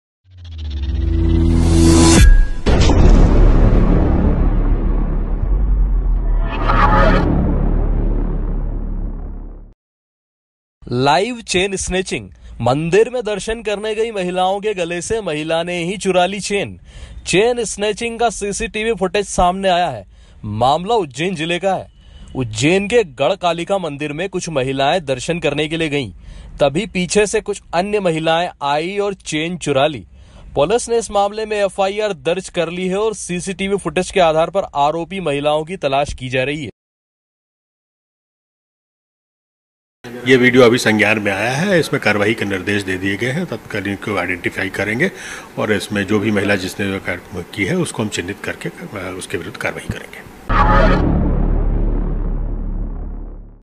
लाइव चेन स्नेचिंग | The Xpose Today News
लाइव चेन स्नेचिंग। मंदिर में दर्शन करने गई महिलाओं के गले से महिला ने ही चुरा ली चेन । चेन स्नेचिंग का सीसीटीवी फ़ुटेज सामने आया है। मामला उज्जैन के गढकालिका मंदिर का है। पुलिस ने एफ़आइआर दर्ज कर ली है और सीसीटीवी फ़ुटेज के आधार पर आरोपी महिलाओं की तलाश कर रही है।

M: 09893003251